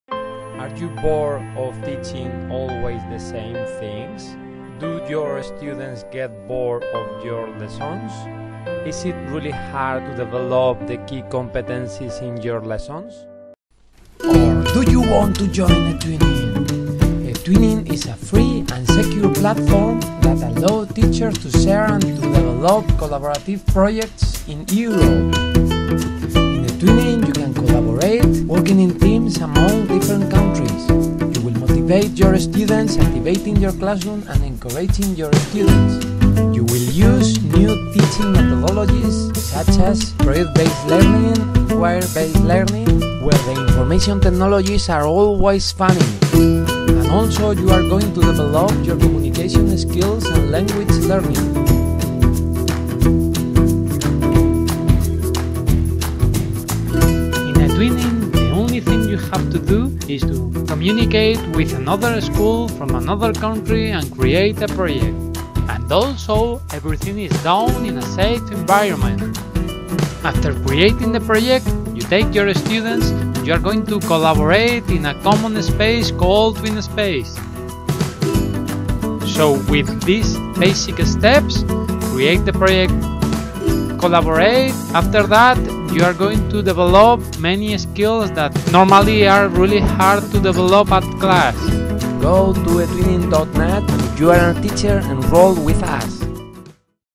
What is etwinning? with sketch notes
What is etwinning? This is a remake of one of my old videos, using my sketchs to explain with some drawings haw easy is to collaborate with other schools from Europe.
Pictures: Classroom is public domain, CC-BY Bored dog by Benoit Dupont & Board is public domain.